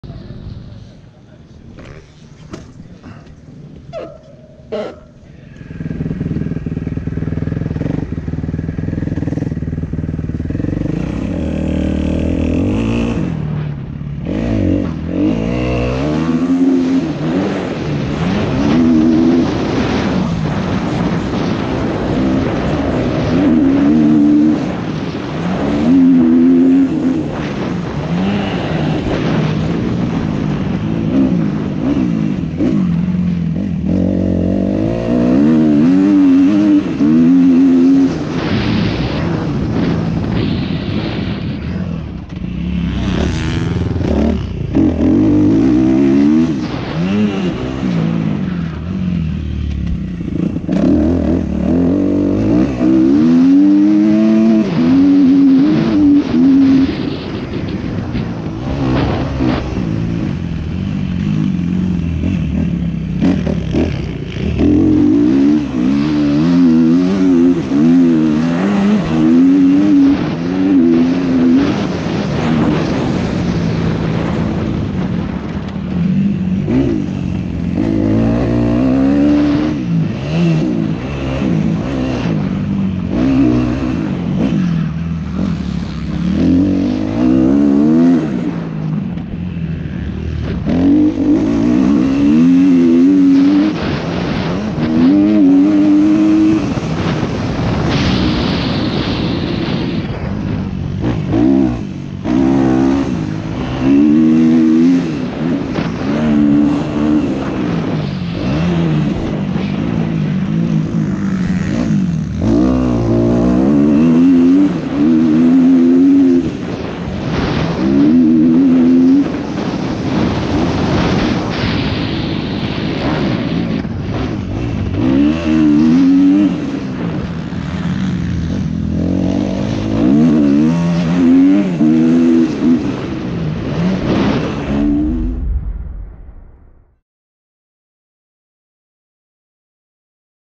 Caresana MX Park: Trackcheck 2018
Motocross GoPro Trackcheck: Caresana MX Park in Ottobiano, Italy. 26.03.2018